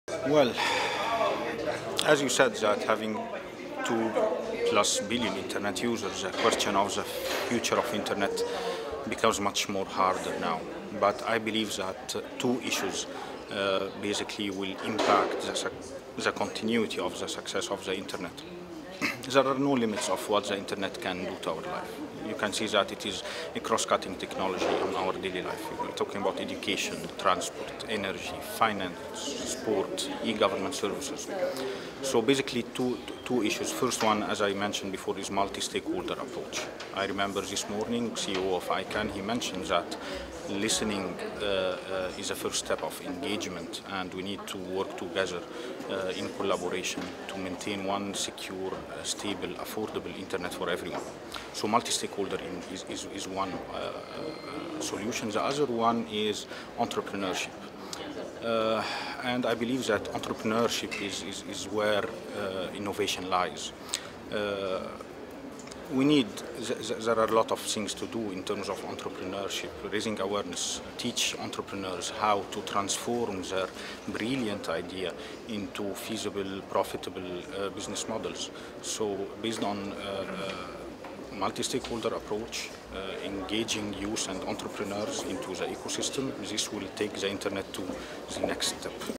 Hope for the Future of the Internet - WTPF and WSIS 2013: George Victor Salama
George Victor Salama, Senior Manager for Public Policy, Samena Telecommunications Council, United Arab Emirates, shares an answer to the question, "What is your greatest hope for the future of the Internet?" during the 2013 World Technology Policy Forum and 2013 World Summit on the Information Society meetings in May in Geneva, Switzerland. Recorded by the Imagining the Internet Center, an initiative of Elon University, North Carolina, USA.